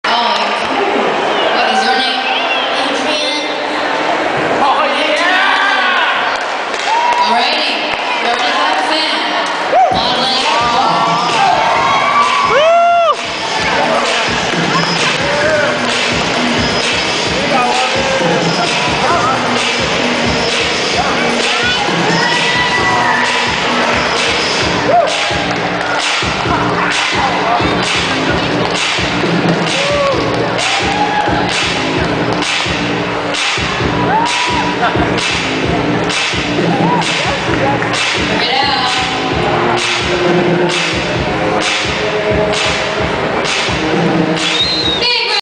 WEM 2007 Audition
Adrian won the WEM 2007 Back to school fashion show for the second time